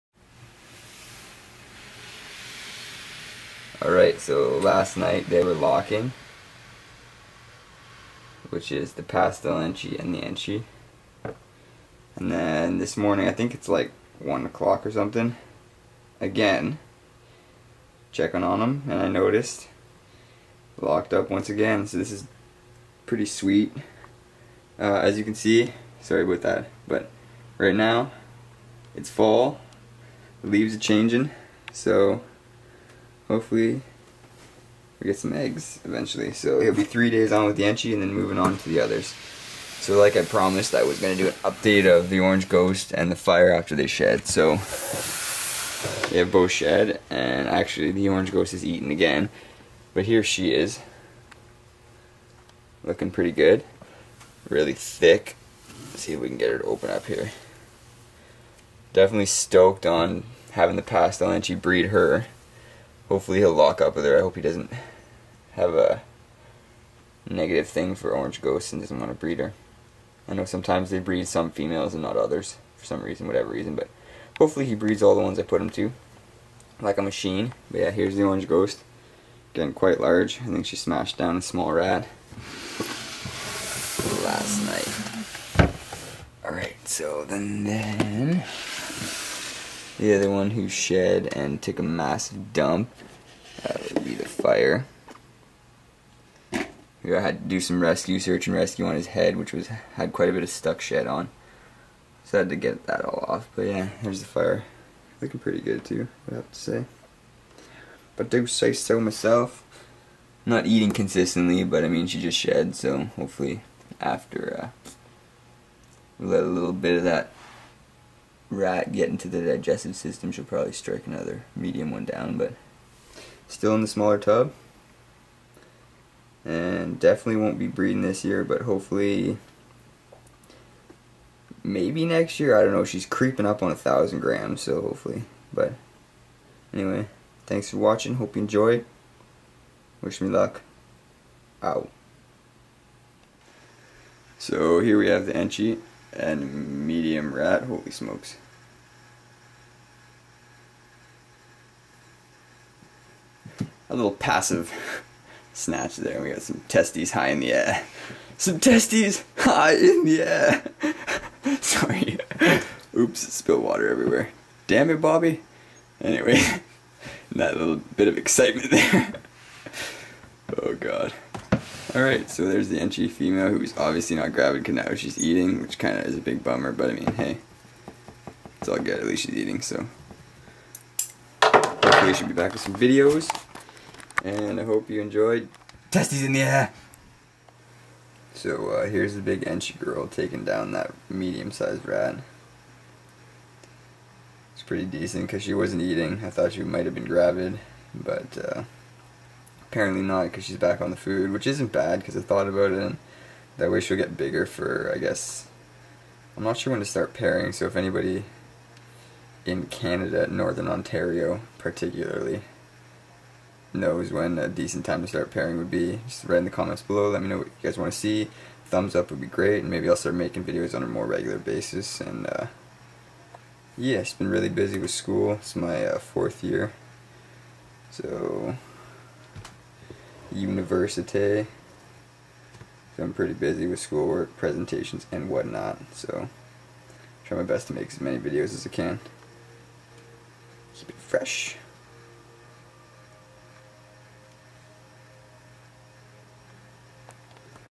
Early Lock and Rest of Ball Python Collection
Early ball python lock between the pastel enchi and enchi and rest of my collection. Also keep watching for the feeding clips at the end!